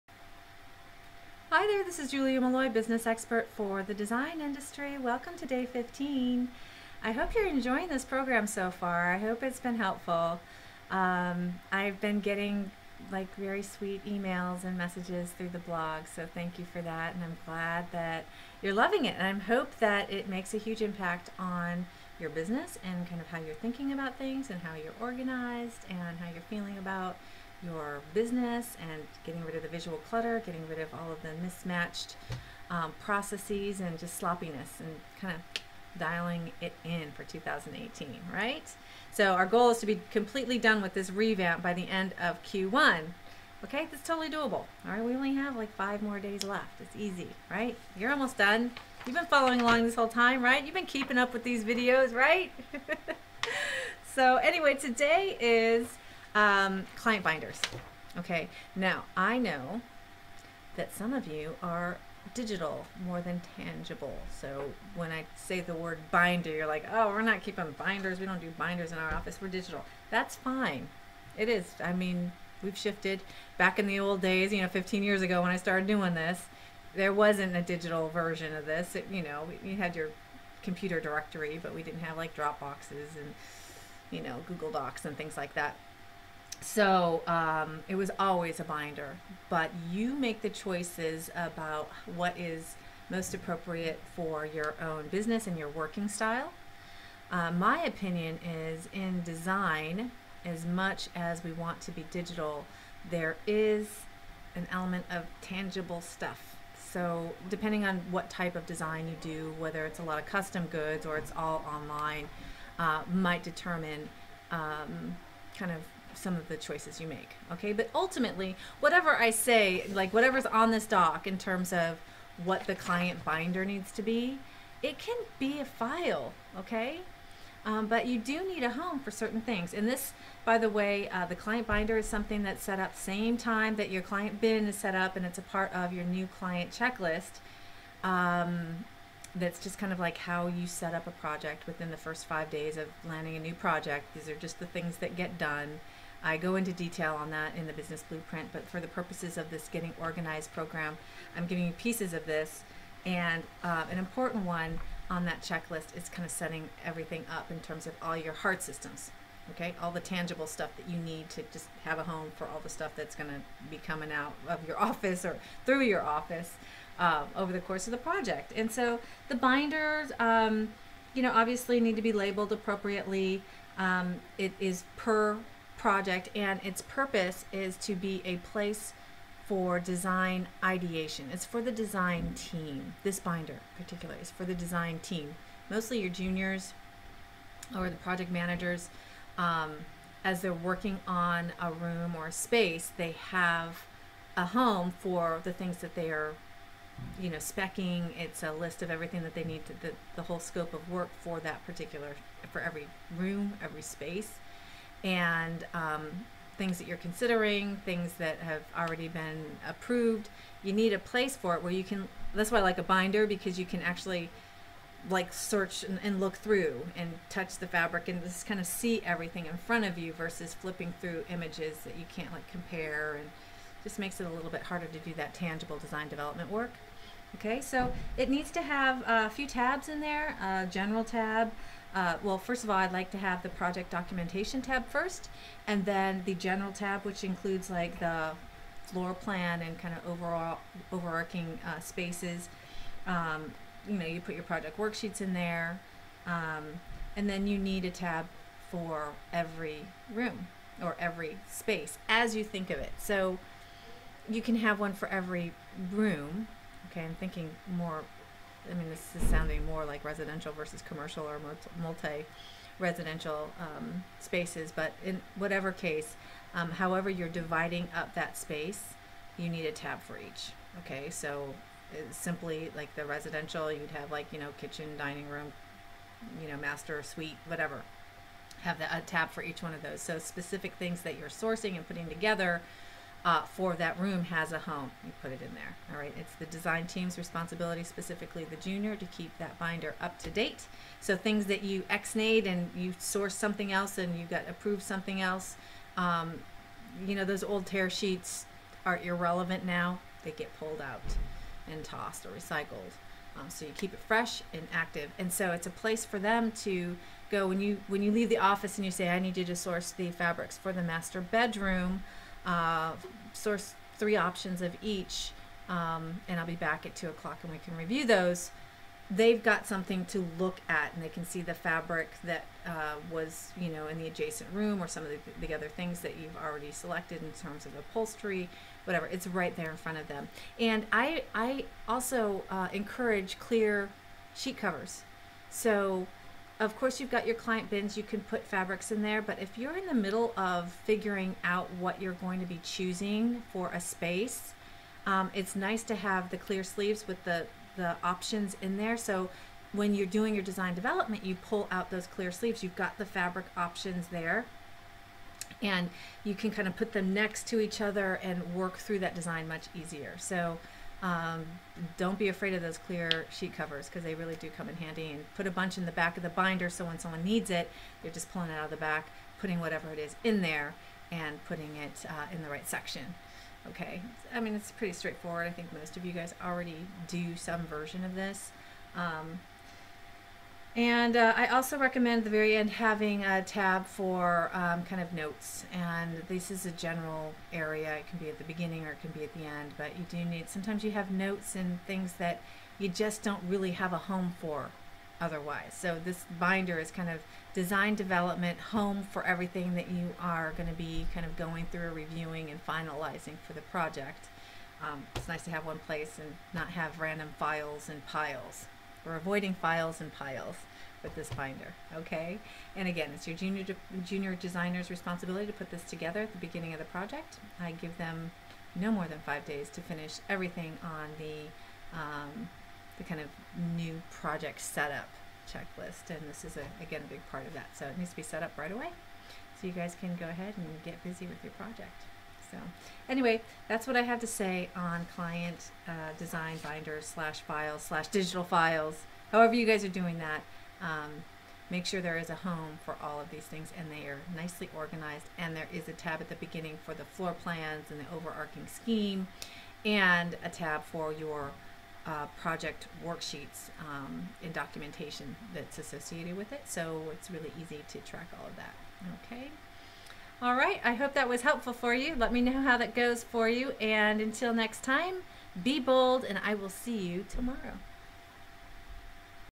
Day 15 - GET ORGANIZED with Julia Molloy - Project Binders
Interior Design Business Expert shares her guidance on using binders for each of your active design projects